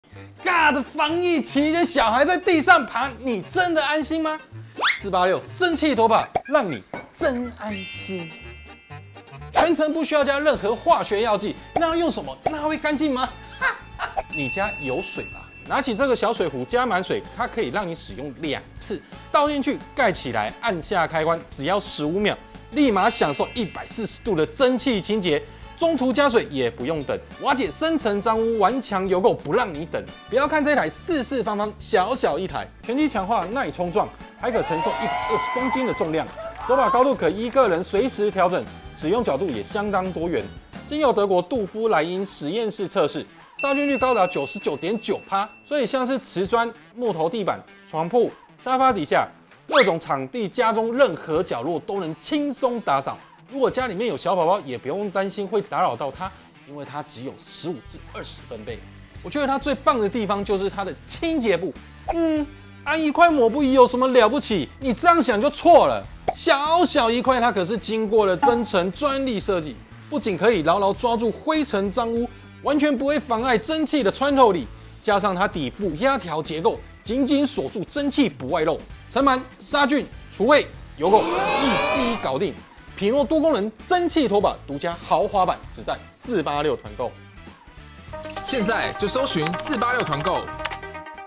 486蒸汽拖把 蒸的！比較安心
不需加任何化學藥劑，幫你家地板99.9%殺菌，連廚房油垢都可以。
PINOH多功能蒸汽清潔機，486獨家甜甜價$2,990，加碼再送4包超導清潔布(含標配共5包)：